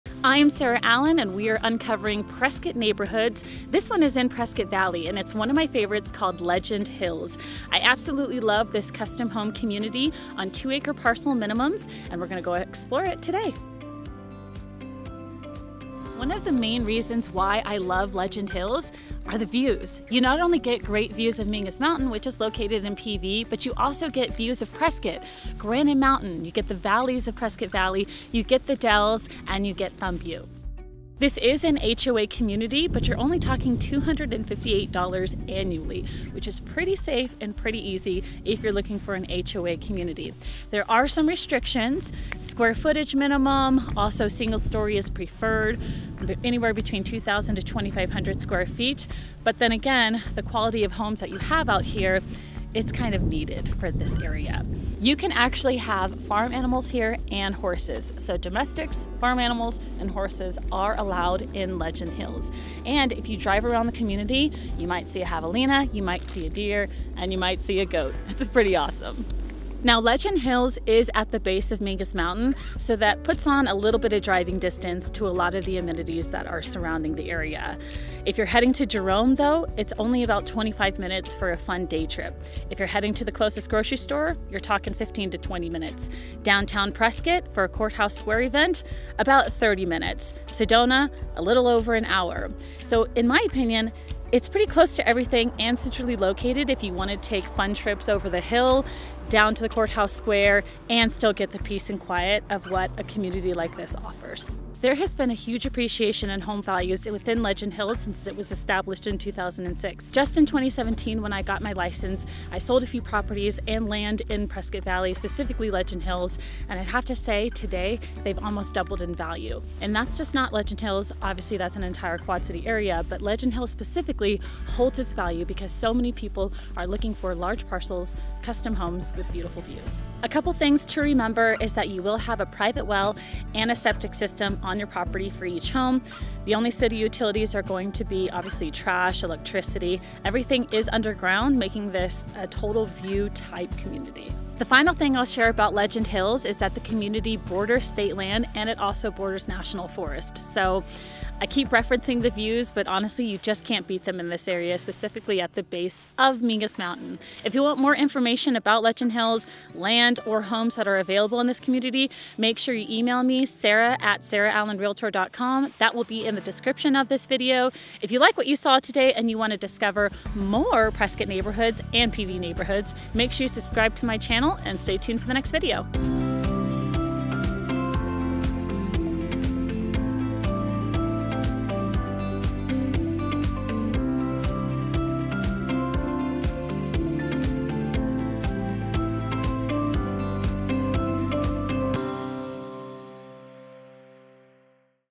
Discover Legend Hills in Prescott Valley, AZ
Legend Hills is a gorgeous community at the base of Mingus Mountain in Prescott Valley, AZ. It features large custom homes in 2+ acre lots with stunning views of Mingus, the Bradshaws, The Dells, Granite Mountain, and Prescott Valley.

Check out homes for sale in Legend Hills on my PrescottValley